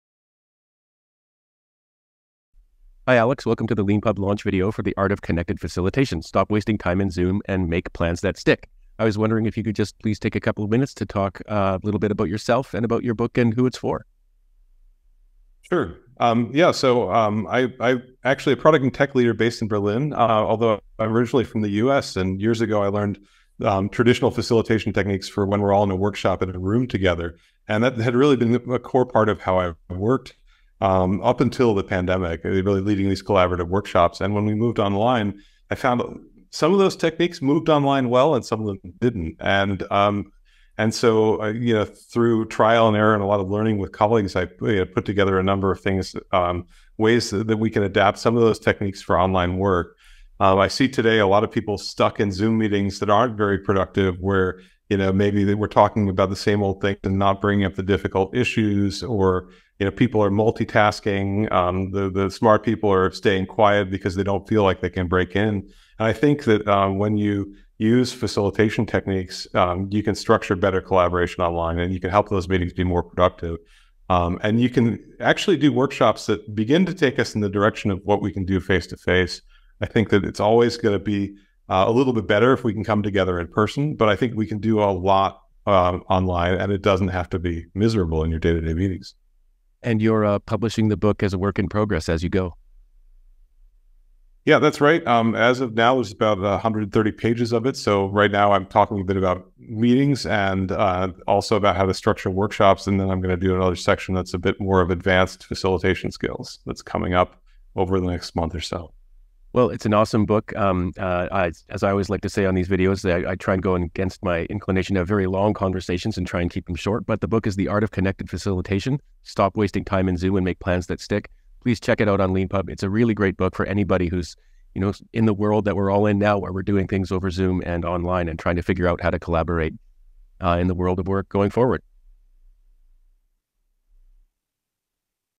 043 Leanpub book LAUNCH! The Art of Connected Facilitation by Alex Pukinskis
Welcome to the Leanpub Launch video for The Art of Connected Facilitation: Stop Wasting Time in Zoom and Make Plans that Stick by Alex Pukinskis!

How much of this week did you spend in online meetings? How much of that time were you listening to the same old perspectives from the same people? Could you have predicted before the meeting who would talk and who would stay quiet?

Did the conversation race from topic to topic without reaching decisions or actions?

Did you watch as people began to multitask, their faces flashing different colors as they worked in email or Slack with Zoom in the background?

Did you sense that the smart, quiet ones were staying quiet because they couldn’t find a way to break in before time was up?

Do you wish your group could just somehow make a decision?

Do you feel like that potential for real collaboration is sitting right below the surface?

For leaders of teams that aren’t in the same room, this book introduces connected facilitation skills. Unlike other facilitation books, we do not assume remote or in-person work is better but rather accept the reality of both.

You'll learn:

- Why we need online facilitators, even when many are returning to the office
- How to improve your weekly team meetings and decision meetings
- Why we hate structured work, and why we need it
- How to design remote workshops
- Why deep collaboration is harder online
- How facilitation changes online compared to in a room
- How to manage the online 'space' using everyday or specialized software.
- How to handle mixed / hybrid workshops
- Power moves for remote facilitators

If you're new to facilitation, this book will get you started. If you're a seasoned 'in the room' facilitator, this book will give you a framework to approach online work that's less fatiguing and brings better results.

Alex has been facilitating collaborative workshops for over 17 years. He's been a computer builder, a customer support rep, a web developer, a software engineer, an agile coach, a product manager, and portfolio manager. He's worked in management consulting and product leadership. He loves hiking, kayaking, and discovering new spaces, and lives in Berlin with his wife and two American cats.

Create Your Own Leanpub Book!

Here's the tutorial showing how to write and publish a Leanpub book in your browser (it's free!)